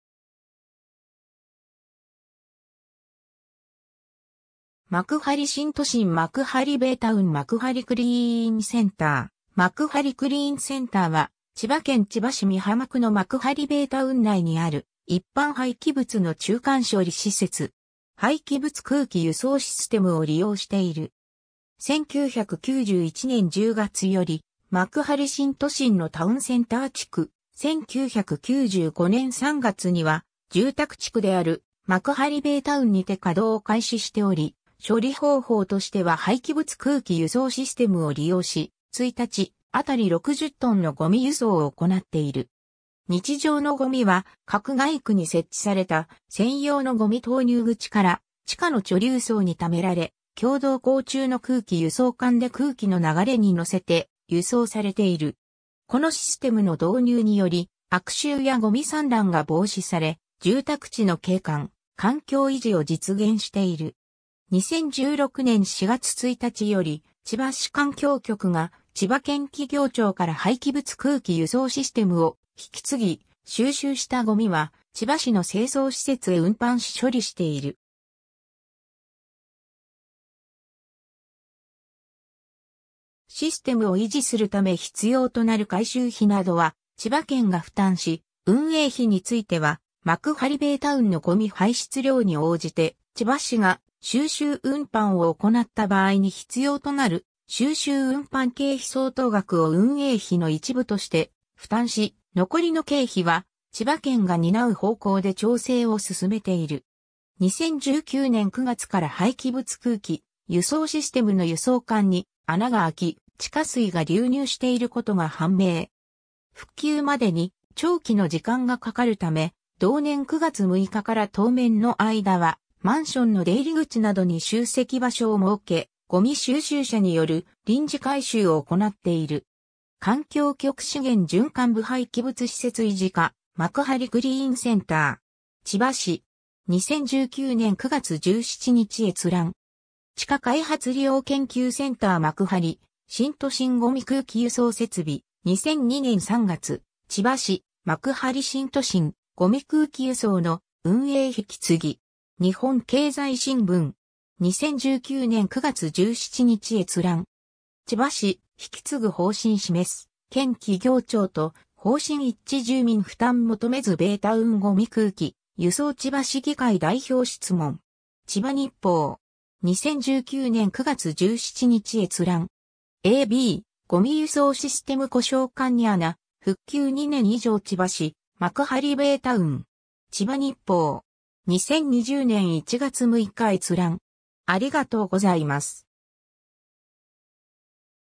幕張クリーンセンター
幕張新都心 」 幕張ベイタウン 」 幕張クリーンセンター 幕張クリーンセンター（まくはりクリーンセンター）は、千葉県千葉市美浜区の幕張ベイタウン内にある一般廃棄物の中間処理施設。
廃棄物空気輸送システムを利用している。
 1991年（平成3年）10月より幕張新都心のタウンセンター地区、1995年（平成7年）3月には住宅地区である幕張ベイタウンにて稼働を開始しており、処理方法としては廃棄物空気輸送システム（家庭ごみを自宅から収集施設に直接送る仕組み）を利用し、1日あたり60トンのごみ輸送を行っている。
日常のゴミは各街区に設置された専用のごみ投入口から地下の貯留槽に貯められ、共同溝中の空気輸送管で空気の流れにのせて輸送されている。
このシステムの導入により悪臭やごみ散乱が防止され、住宅地の景観・環境維持を実現している。
 2016年（平成28年）4月1日より千葉市環境局が千葉県企業庁から廃棄物空気輸送システムを引継ぎ、収集したごみは千葉市の清掃施設へ運搬し処理している。
システムを維持するため必要となる改修費などは千葉県が負担し、運営費については幕張ベイタウンのごみ排出量に応じて千葉市が収集運搬を行った場合に必要となる収集運搬経費相当額を運営費の一部として負担し、残りの経費は千葉県が担う方向で調整を進めている。
 2019年（令和元年）9月から廃棄物空気輸送システムの輸送管に穴が空き、地下水が流入していることが判明。
復旧までに長期の時間がかかるため、同年9月6日から当面の間はマンションの出入り口などに集積場所を設け、ごみ収集車による臨時回収を行っている。
 ^ “環境局 資源循環部 廃棄物施設維持課 幕張クリーンセンター”. 千葉市. 2019年9月17日閲覧。
 ^ 地下開発利用研究センター 幕張新都心ごみ空気輸送設備、2002年3月 ^ “千葉市、幕張新都心「ゴミ空気輸送」の運営引き継ぎ”. 日本経済新聞（2013年12月3日作成）. 2019年9月17日閲覧。
 ^ “千葉市「引き継ぐ」方針示す 県企業庁と方針一致住民負担求めず ベイタウンごみ空気輸送 千葉市議会代表質問”. 千葉日報（2013年12月6日作成）. 2019年9月17日閲覧。
 ^ a b “ごみ輸送システム故障　管に穴、復旧２年以上　千葉市・幕張ベイタウン”. 千葉日報（2019年12月4日作成）. 2020年1月6日閲覧。